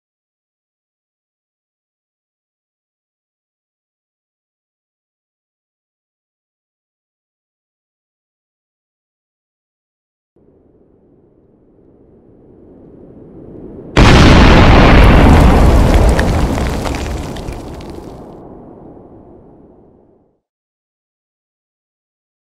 Realistic Explosion with Adobe After Effects
Motion tracked with Mocha, all effects are from "Action Essentials 2" by VideoCopilot.

Effects were toned and masked to fit in, larger explosion has an added glow to it. I tested couple versions of shockwaves but they didn't seem to work as intended.
Curves and vignette as usual.